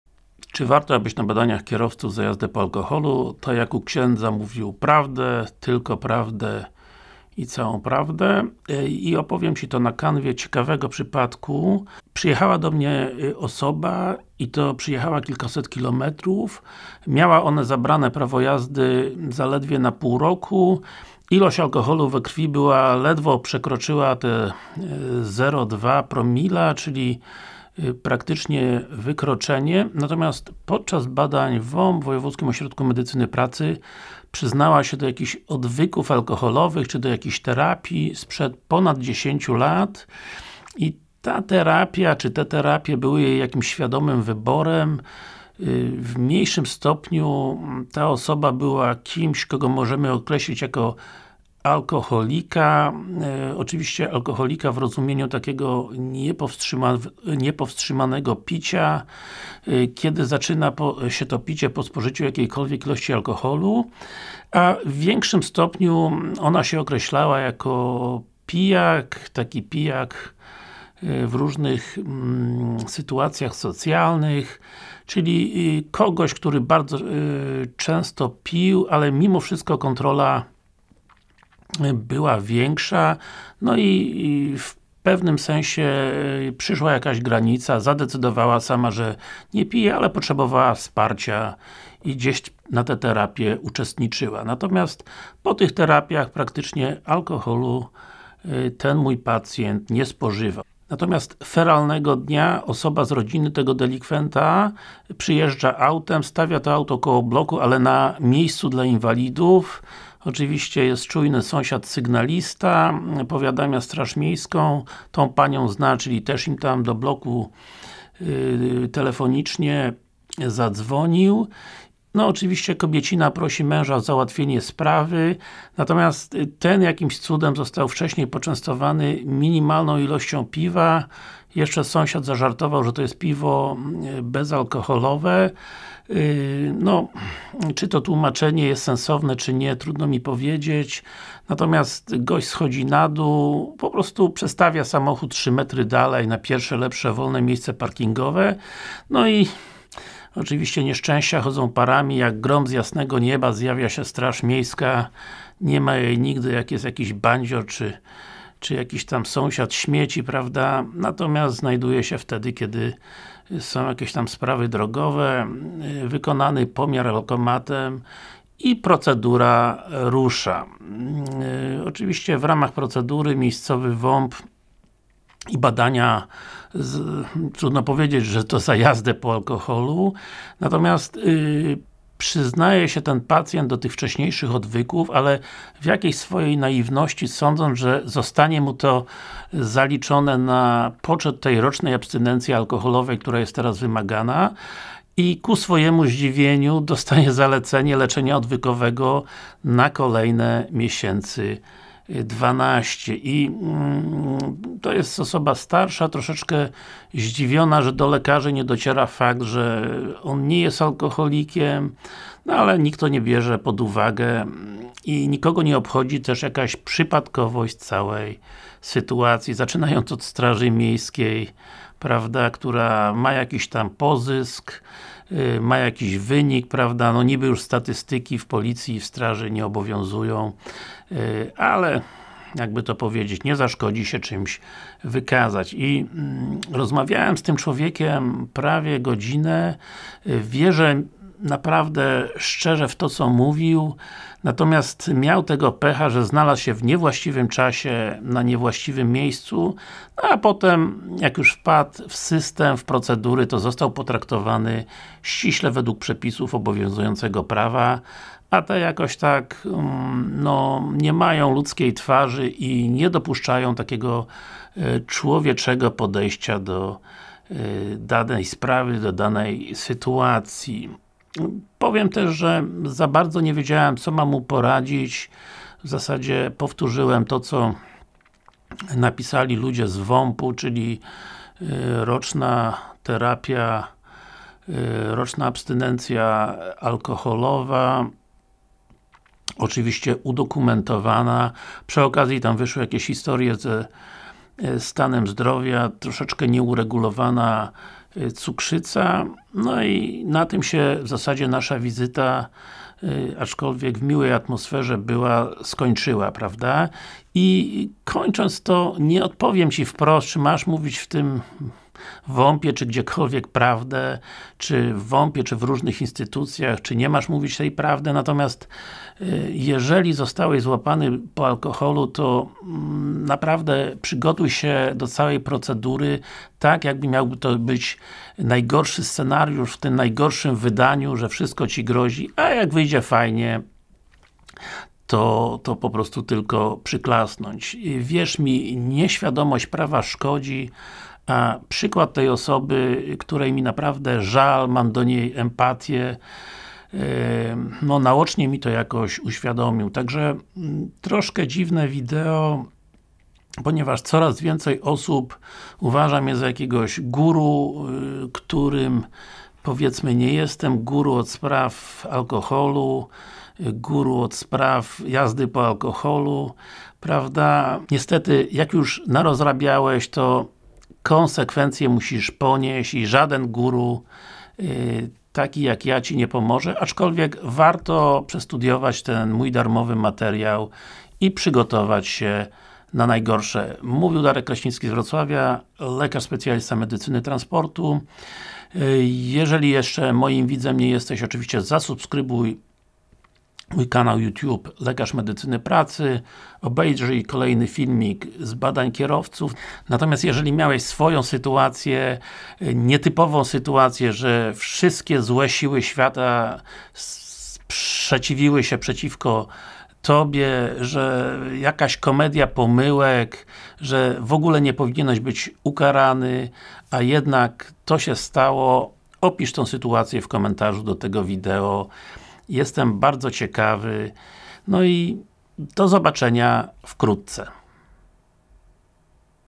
Czy warto mówić prawdę w WOMP?
Czy kłamstwo może być lepsze od prawdy?

Czy warto mówić prawdę w WOMP na badaniach kierowców za jazdę po alkoholu?